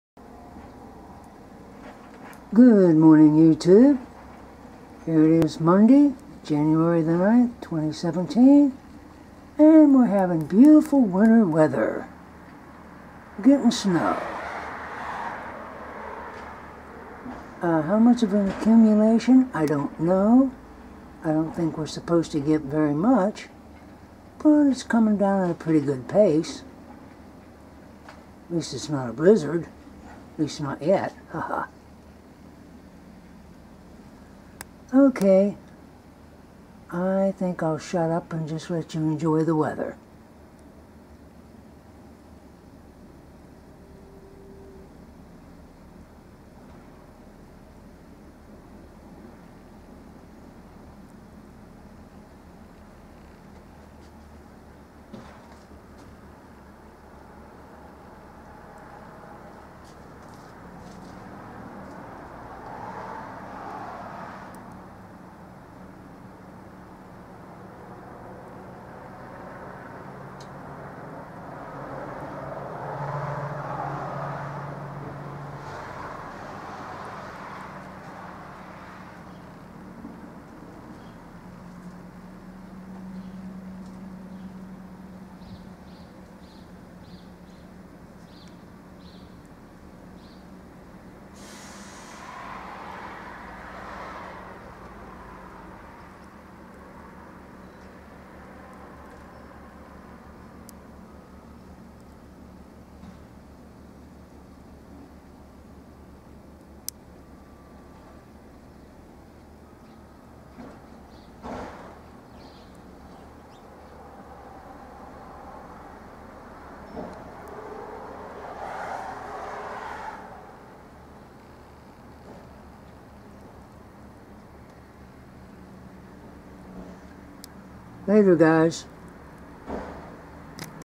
Snowfall Monday Jan. 9, 2017
Light snowfall came through our area, not sure how much we'll get. I'll update it if we get more.